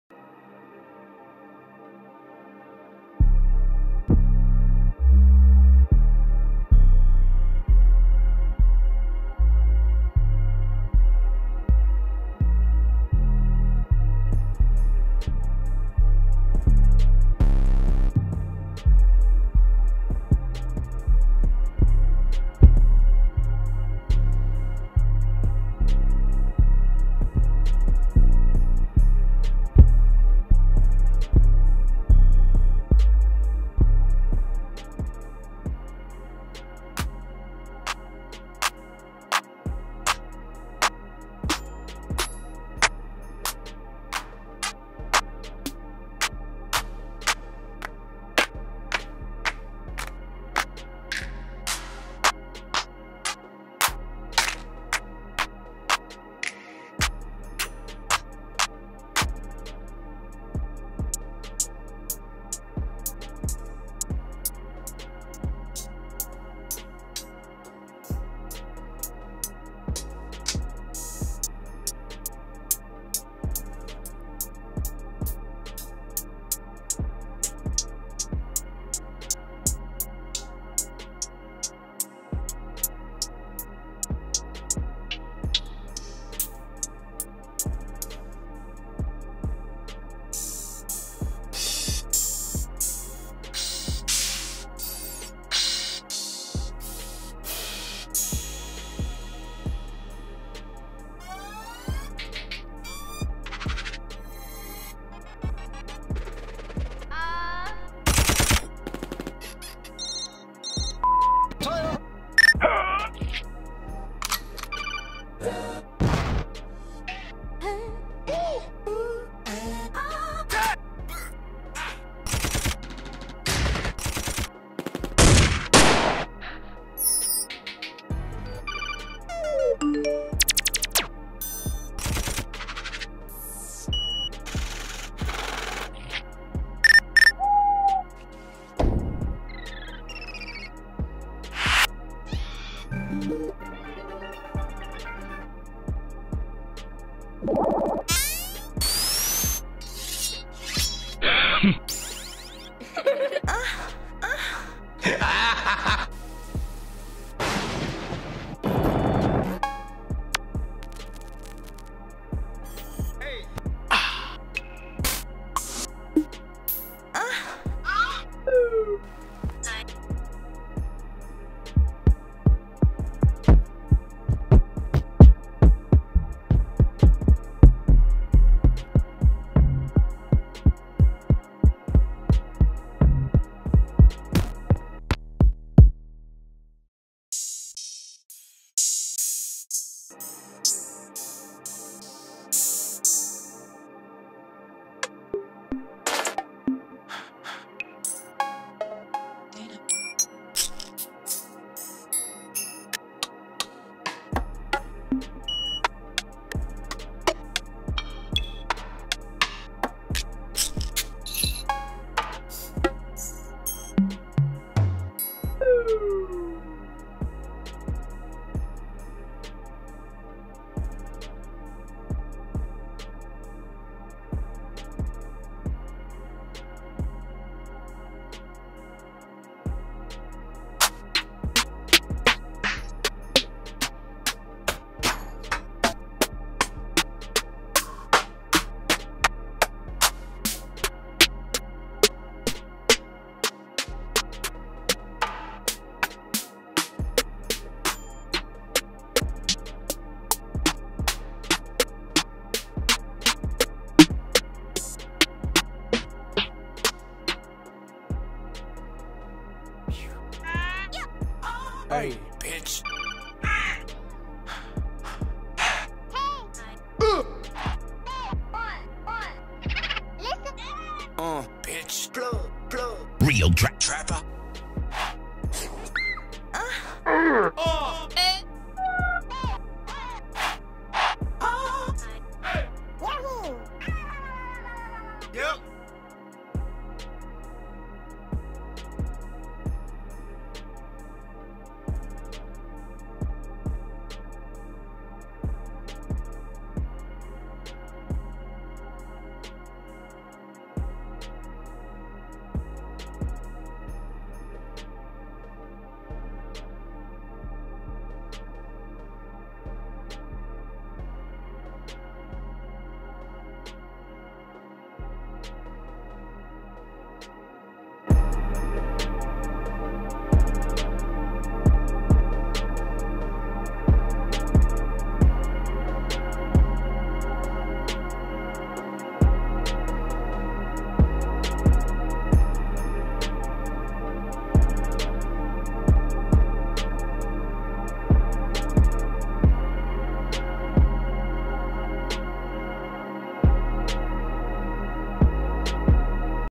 ICYTWAT DRUM KIT BY SMOKKESTAXKK | ETHEREAL DRUM KIT / LORD FUBU DRUM KIT / DIVINE COUNCIL DRUM KIT
🌊 🥁 PURCHASE ICYTWAT DRUM KIT OUT NOW!

First of all, I created this kit for myself, I literally use everything that I share with the world

This is a great kit for both beginners and pros, as it has enough content to keep your beats sounding different and original everytime.